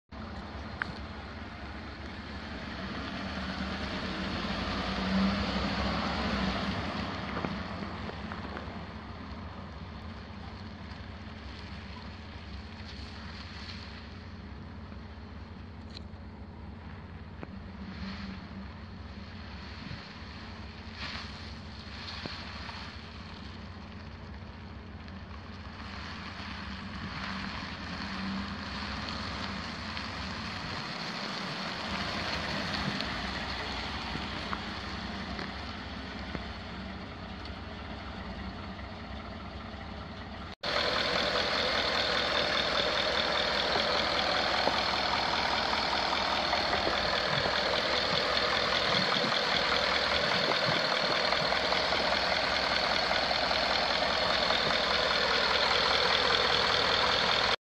2013 Ford F150 XL pickup truck for sale at auction | bidding closes February 8, 2022
2013 Ford F150 XL  pickup truck
 Miles: 230,197 on odometer
 VIN: 1FTNF1EF1DKG49128
Engine
 Displacement: 5.0L V8 OHV 16V
 Cylinders: 8
 Fuel type: Gas
Transmission
 Automatic
 Four wheel drive
Interior
 AC, Heat
 Cruise control
Tires
 Size: 245/75R17
Notes
 Dash warning indicator: Check engine
Kansas title
It's located in Talmage, KS, and we're selling it to the highest bidder regardless of price.